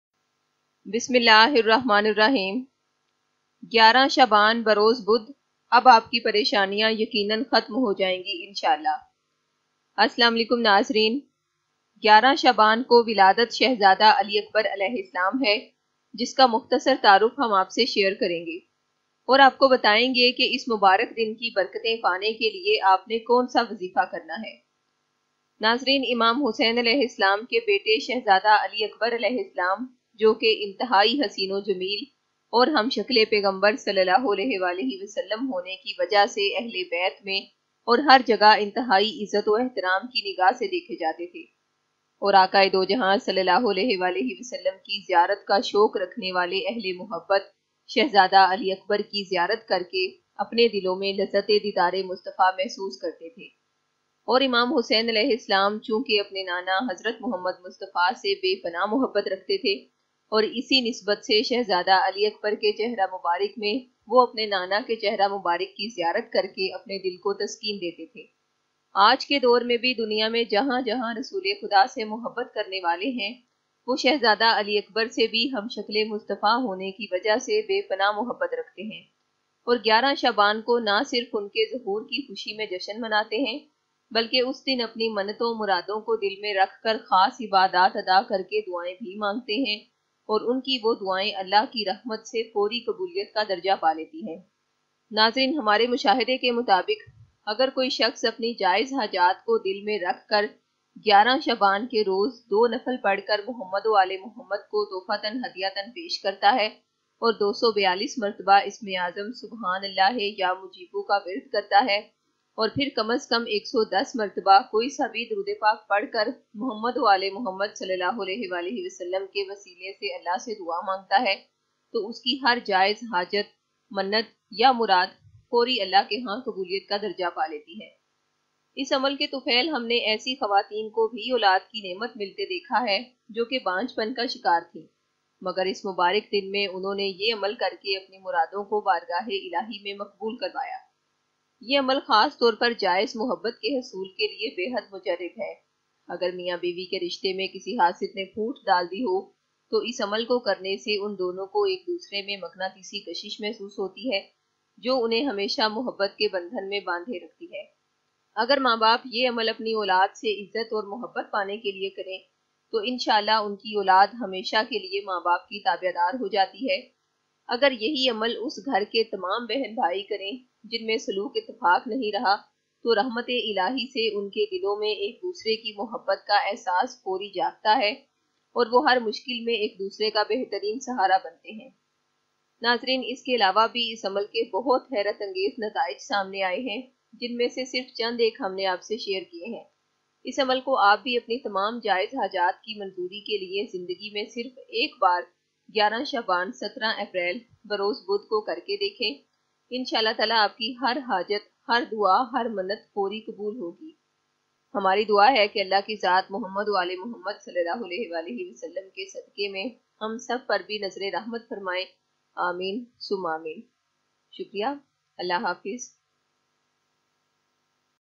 11 Shaban 2019 Ibadat - Powerful Amal - Wazifa - Nafal - Har Pareshani Khatam - Har Dua Fori Qubool
Aaj ki video mein ham 11shaban ba roz jashan e zahoor / wiladat ShahzadaAliAkbar A.S. ap se har pareshani ko khatam karne aur har dua ko Bargah e Ilahi se fori qubool karwanay ka bohat hi powerfulamal share kar rahe hain. 11 Shaban 2019 ki ye ibadat, wazifa aur nafal agar ap hamaray bataye gaye tariqay k mutabiq parh lete hain to Insha Allah apki har jaiz dua fori qabooliyat kar darja pa lay gi.